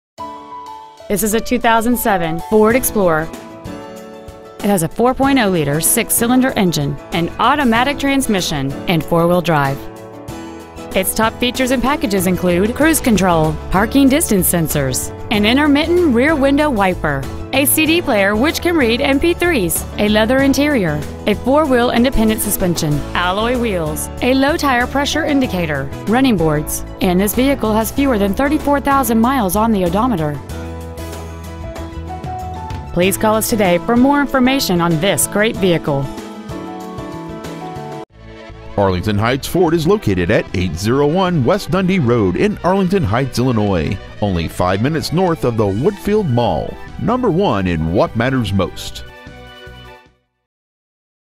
2007 Ford Explorer Mount Prospect IL
We've been honored to serve the Arlington Heights IL area , we promise that your experience at our dealership will exceed your expectations !

 Year : 2007
 Make : Ford
 Model : Explorer
 Engine : V6 4.0 Liter
 Trans . : Automatic
 Exterior : Blue
 Miles : 33,641
 Interior : Beige
 Stock : P12894

 801 W. Dundee Road Arlington Heights, IL 60004

 Preowned 2007 Ford Explorer Arlington Heights IL